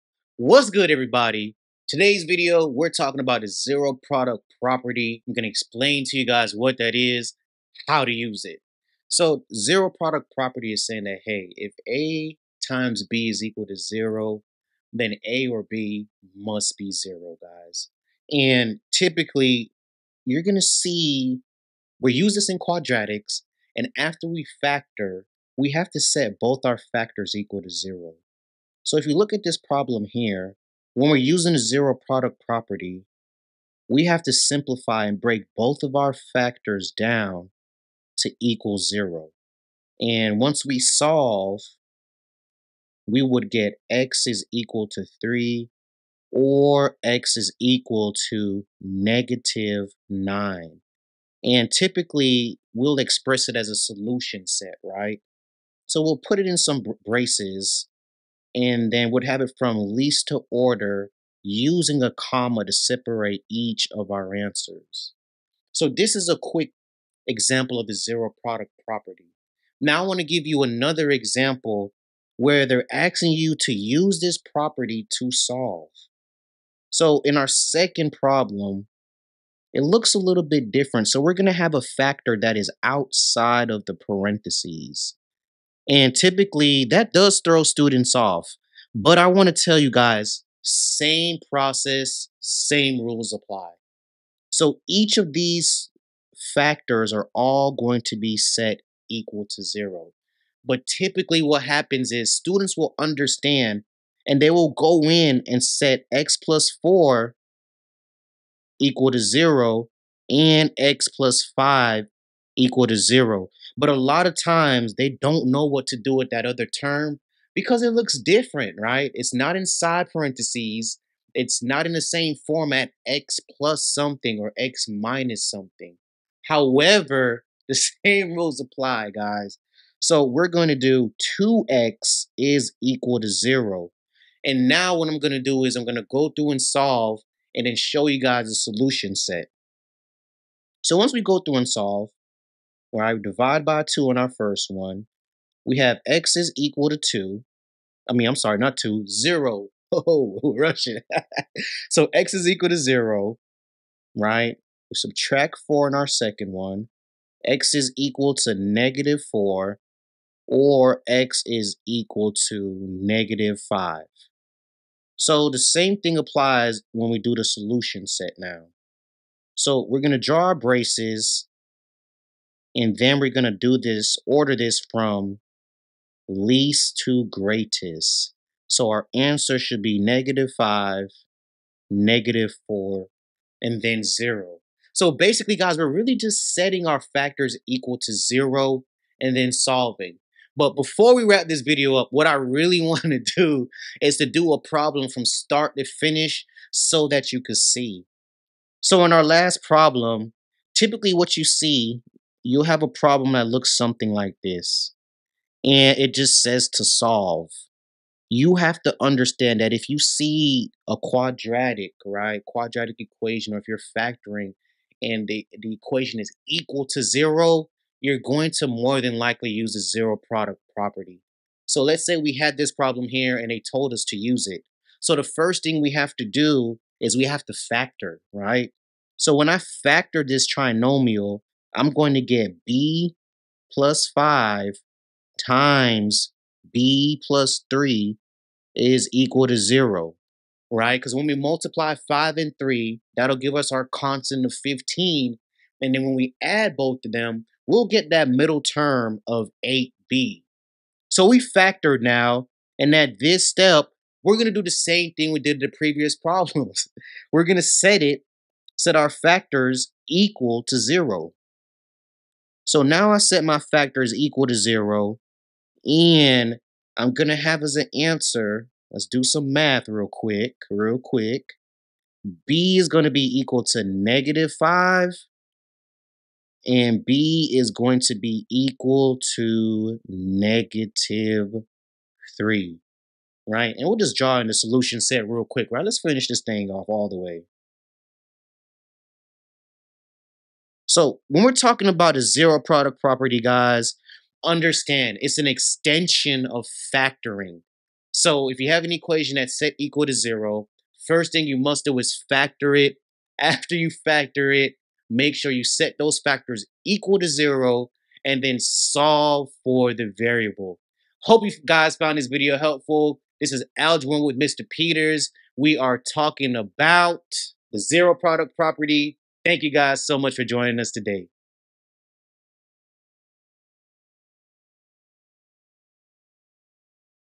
Solving Quadratics with the Zero Product Property
Today's review goes over the Zero Product Property. In this video we will explain what the Zero Product Property is, When to use it, and How to properly use it. Click the video above for our tutorial on the Zero Product Property!